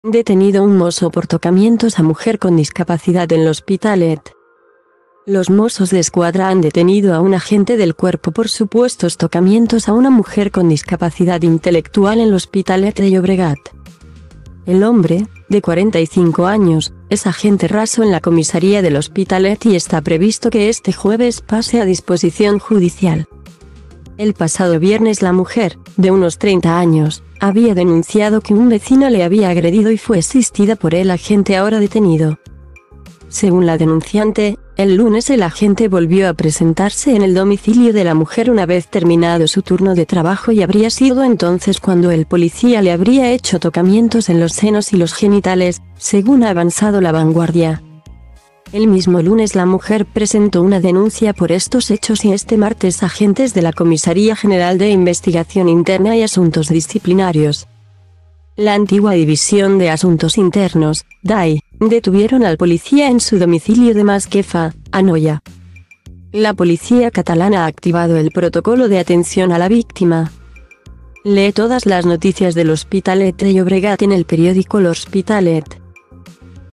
Detenido un mosso por tocamientos a mujer con discapacidad en L'Hospitalet
Los Mossos d'Esquadra han detenido a un agente del cuerpo por supuestos tocamientos a una mujer con discapacidad intelectual en L'Hospitalet de Llobregat. El hombre, de 45 años, es agente raso en la comisaría de L'Hospitalet y está previsto que este jueves pase a disposición judicial. El pasado viernes la mujer, de unos 30 años, había denunciado que un vecino le había agredido y fue asistida por el agente ahora detenido.